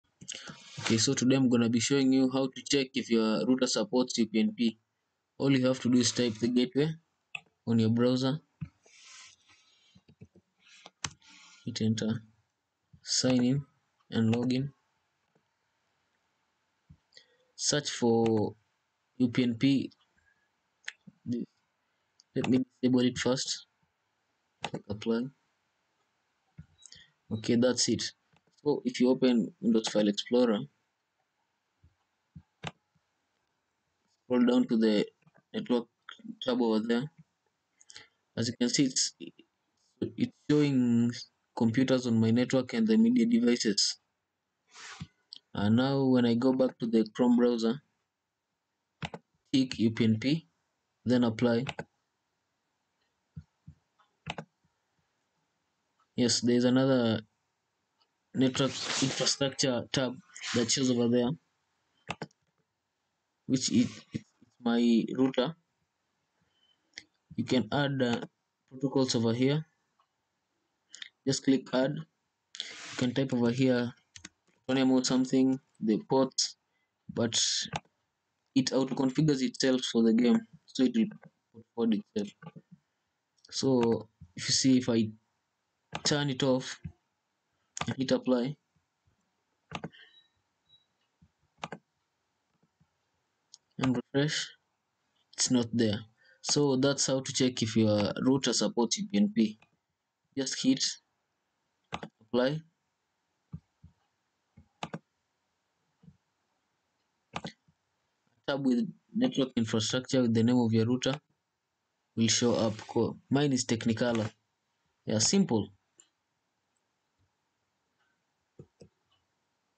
Check if router supports upnp for plutonium e.t.c
If you enable upnp in your router and doesnt show up that means either your isp (internet service provider) does not support it or offer it you may try contacting them and ask if it possible.